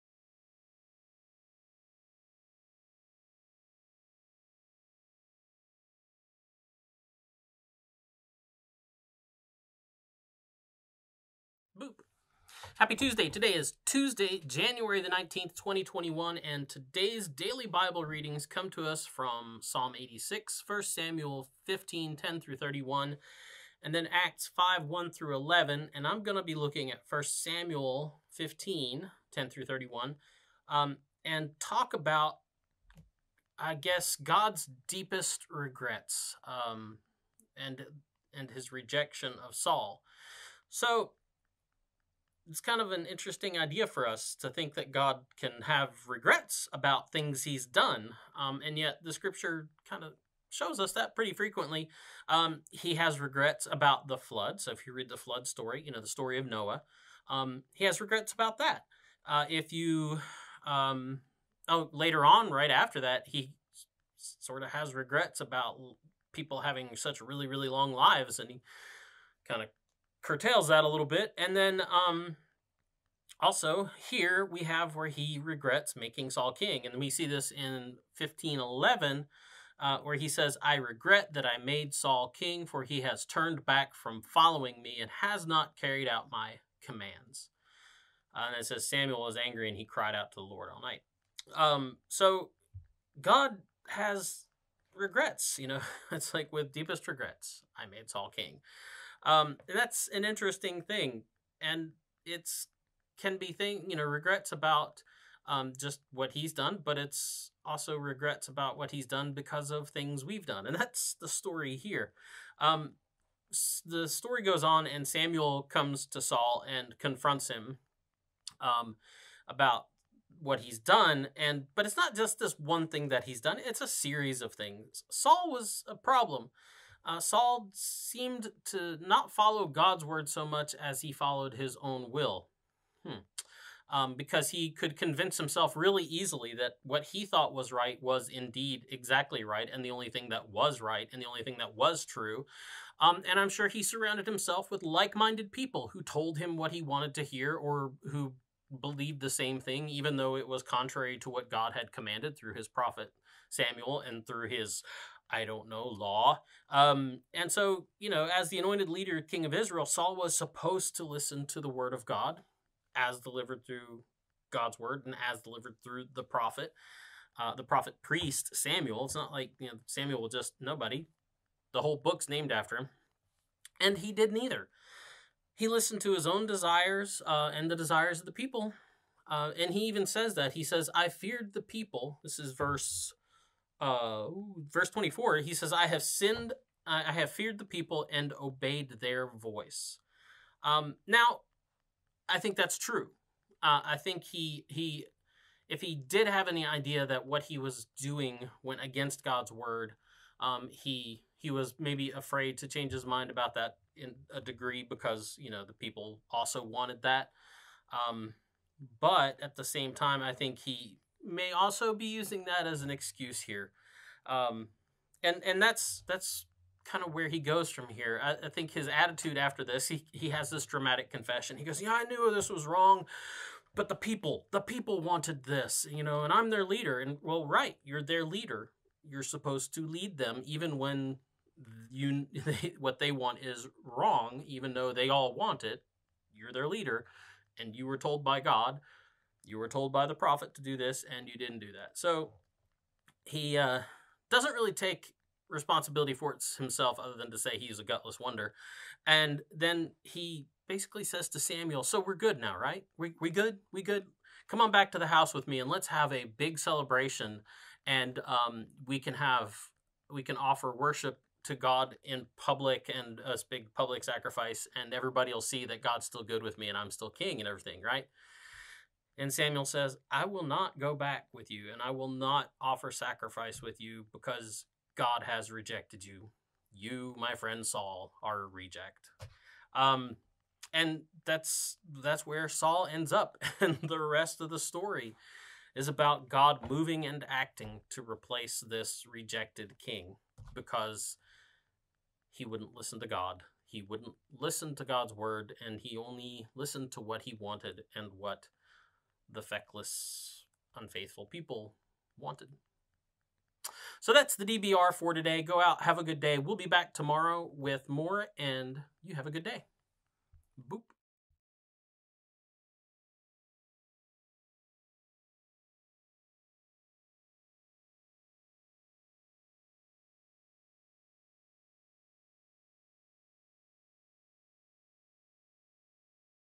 Daily Bible Reading for January 19, 2021 "Reject"
Today in Wesley’s DBR on YouTube, God has deep regrets. “Reject”

Daily Bible Readings for Tuesday, January 19, 2021: Psalm 86; 1 Samuel 15:10-31; Acts 5:1-11